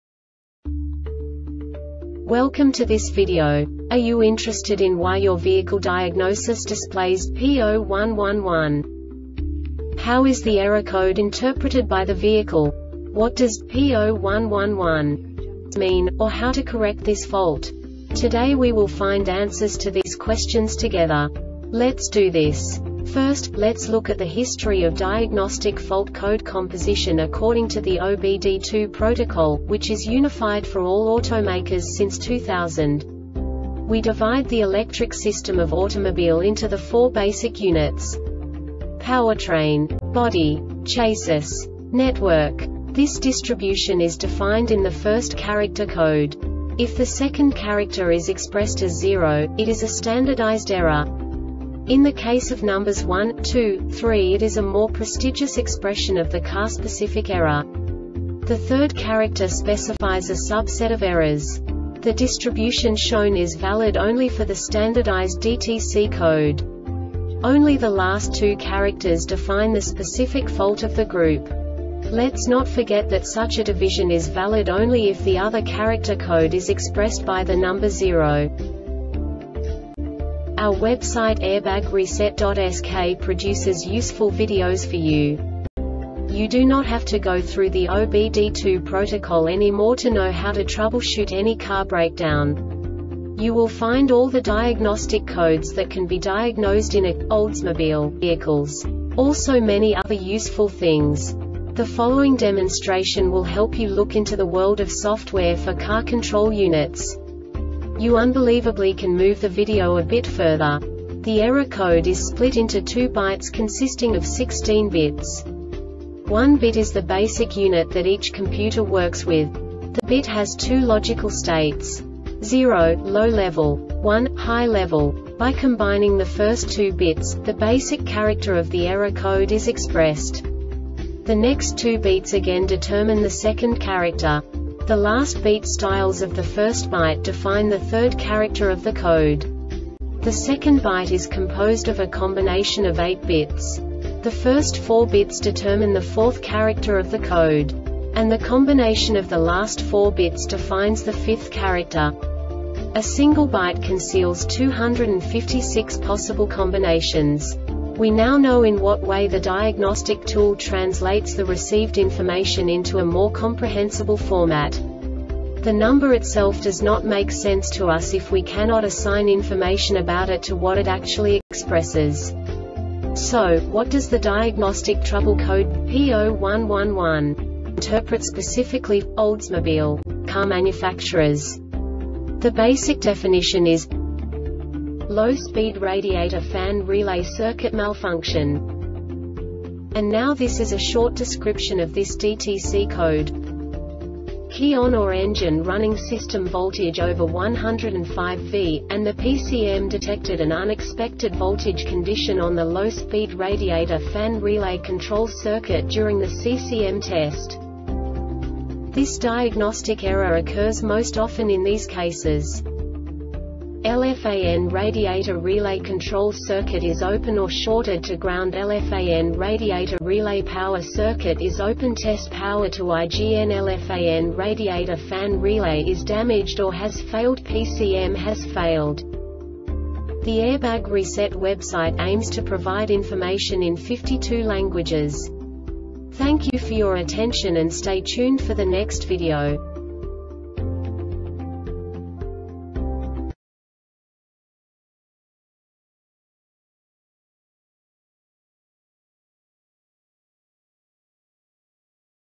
DTC Oldsmobile P0111 Short Explanation
Contents:
0:21 Basic DTC analysis according to OBD2 protocol standard.
1:48 Insight into programming
2:58 A diagnostically understandable description of the Diagnostic Trouble Code
3:05 Basic error definition

Make:
Oldsmobile
Code:
P0111
Definition:
Low Speed Radiator Fan Relay Circuit Malfunction
Description:
Key on or engine running;system voltage over 10.5v,and the PCM detected an unexpected voltage condition on the Low Speed Radiator Fan Relay control circuit during the CCM test.
Cause:
LFAN radiator relay control circuit is open or shorted to groundLFAN radiator relay power circuit is open (test power to IGN)LFAN radiator fan relay is damaged or has failedPCM has failed